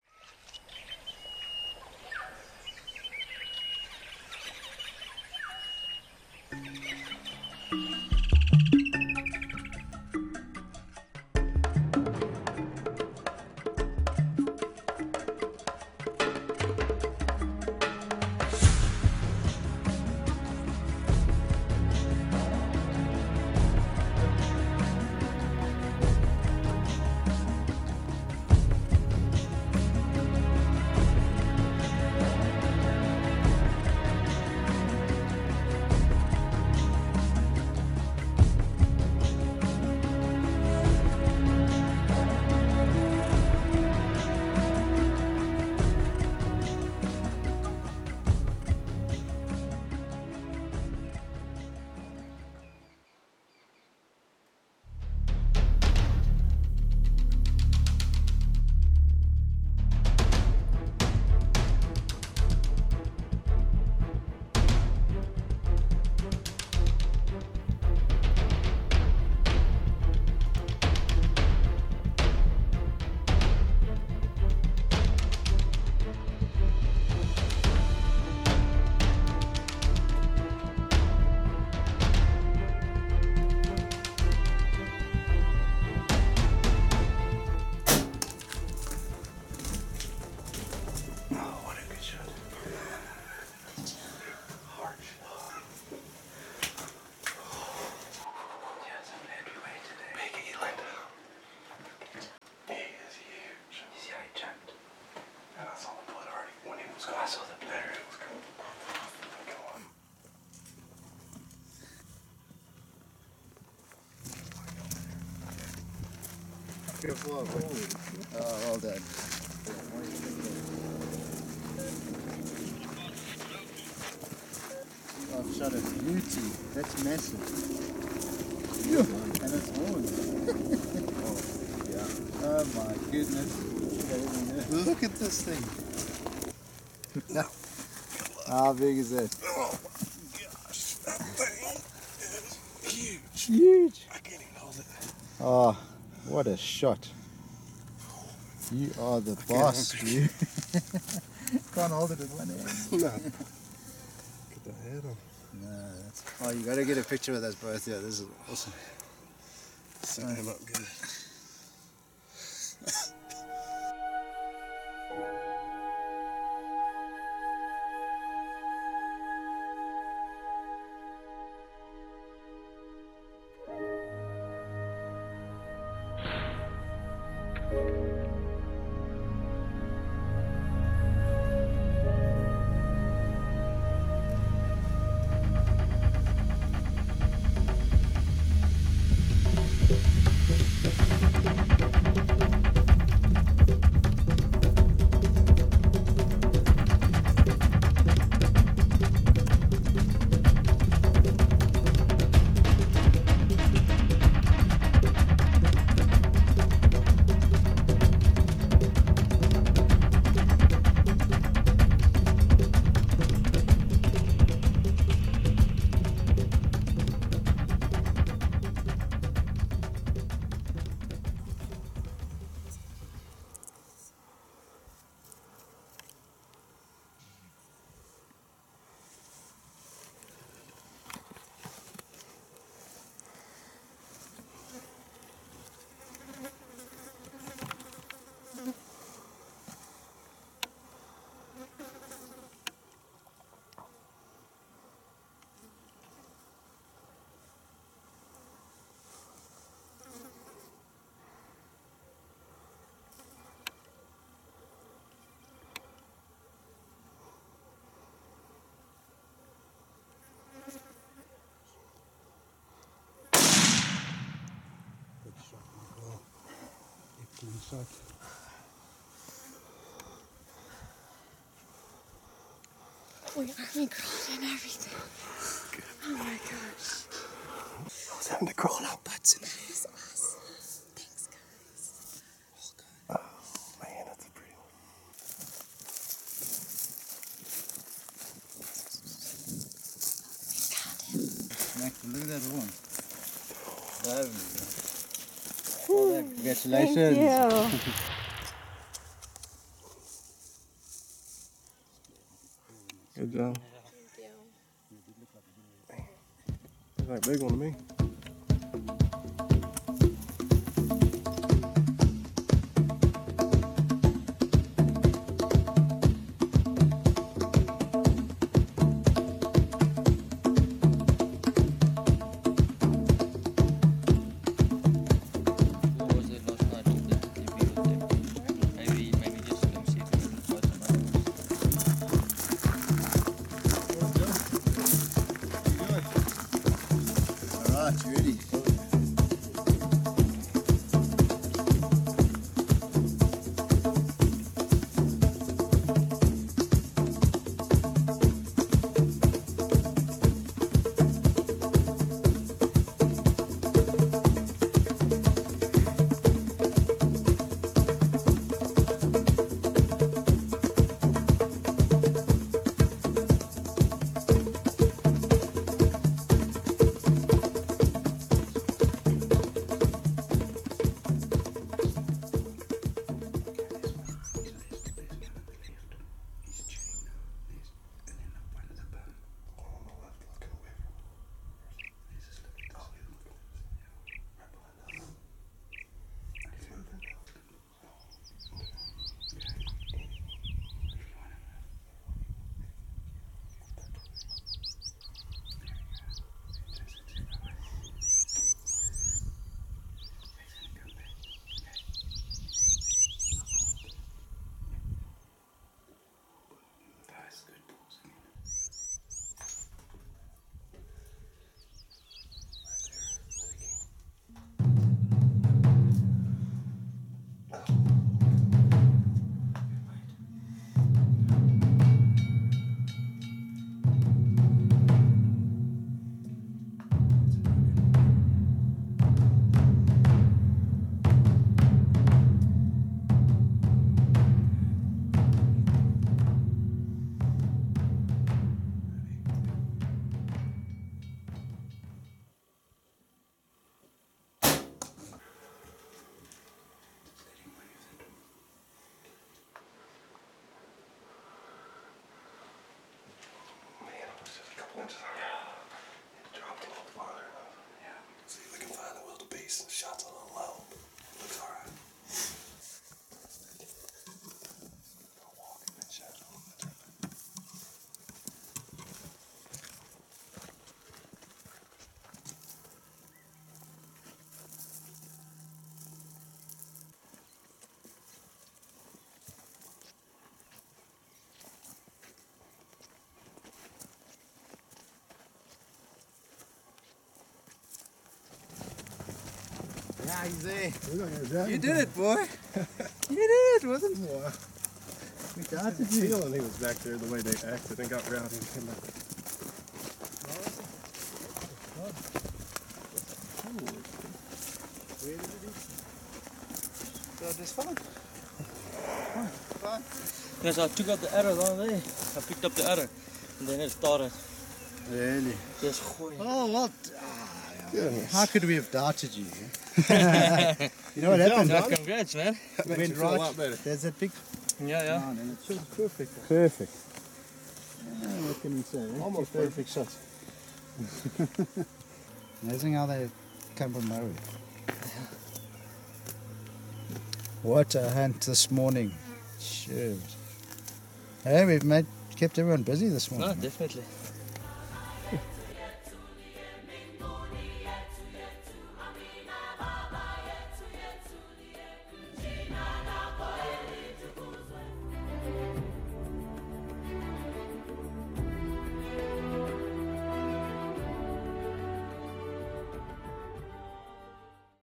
Africa Part 2 - Eland, Blesbok, and Wildebeest
Part 2 of our trip to Africa with Comre Safaris. We took an Eland, Blesbok, and Blue Wildebeest in this second segment of our hunt. We used a Mathews Z7 Extreme for the bow hunts and a 7x57 with 160 grain Hornady ammo for the rifle hunts.